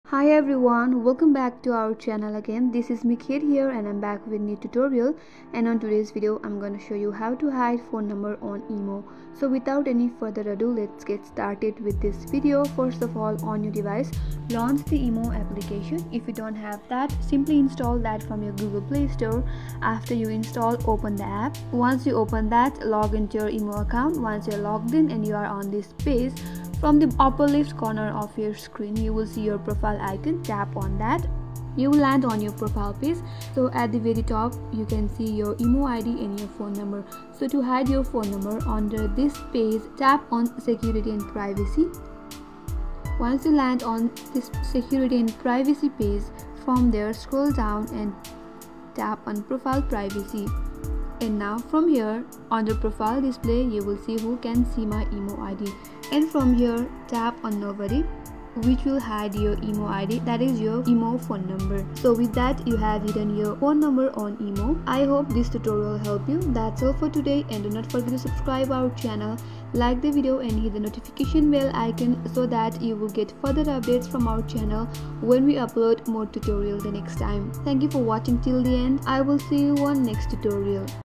How to Hide Phone Number on IMO Account?
Learn, How to Hide Phone Number on IMO Account on your phone with quick and easy steps.

Worried about your privacy on IMO? This Tutorialgurudev guide unlocks powerful ways to protect your information without hiding your phone number. Ditch the limitations and explore expert tips to control who sees your details, secure your messages, and navigate with confidence.